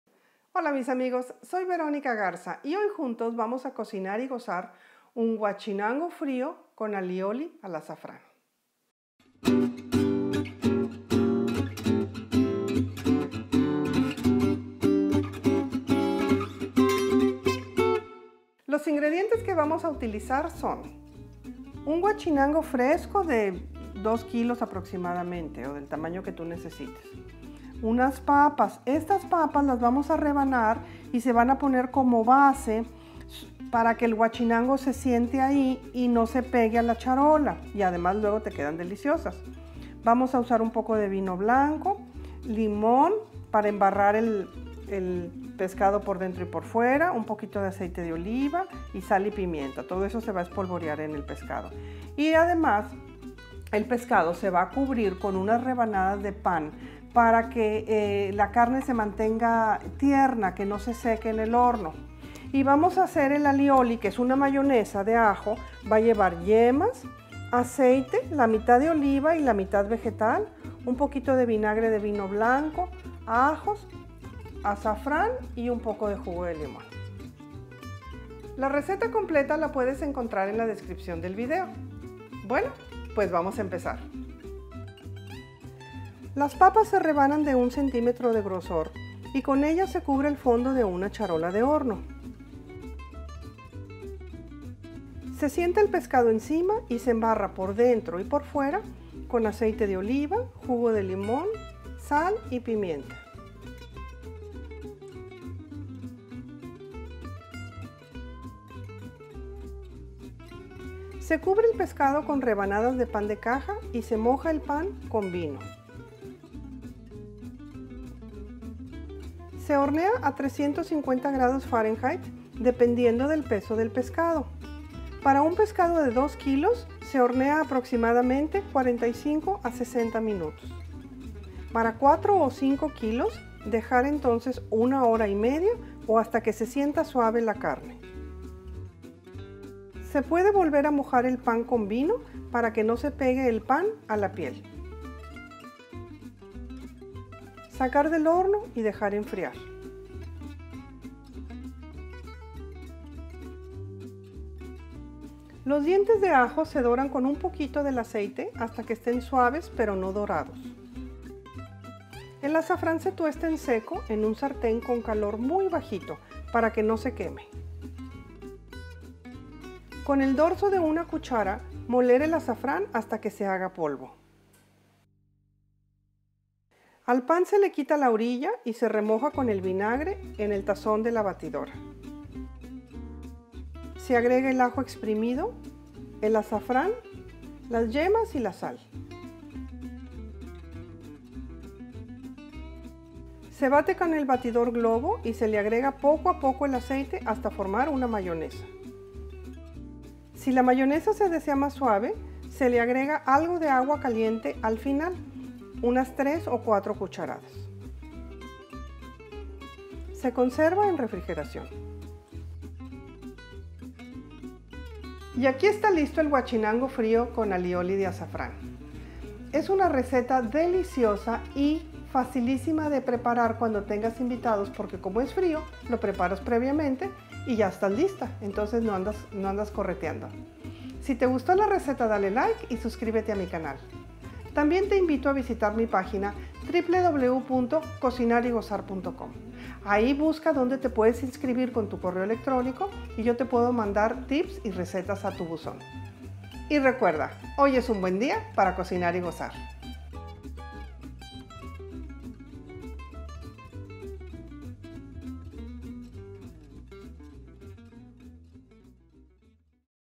Huachinango Frío Con Salsa Aioli Al Azafrán
Receta para preparar huachinango frío con salsa aioli al azafrán. Puedes consultar ingredientes, instrucciones y más recetas en los links.

INGREDIENTES E INSTRUCCIONES

PARA DESCUBRIR MÁS RECETAS VISITA